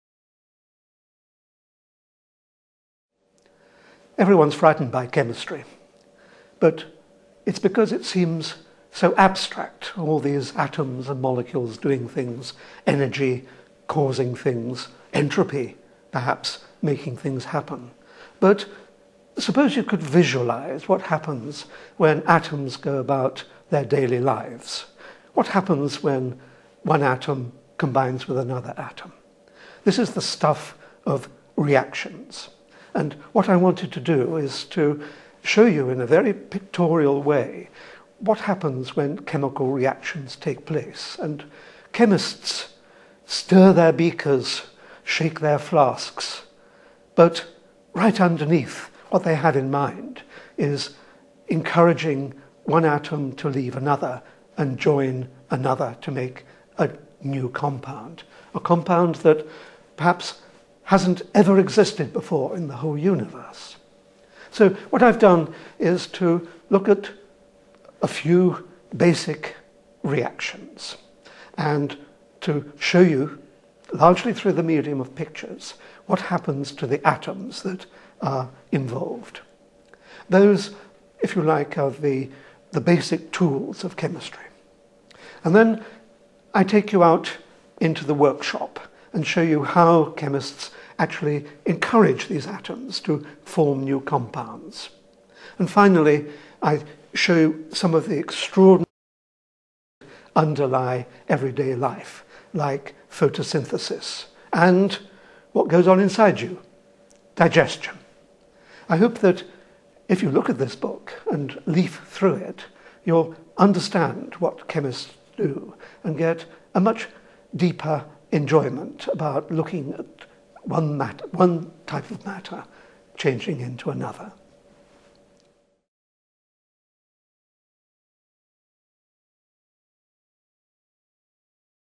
Reactions: The Private Life of Atoms | Peter Atkins
Peter Atkins, author of Reactions, discusses how Chemistry doesn't have to be scary for non-specialists.  Find out how his book can give you the basic tools of Chemistry.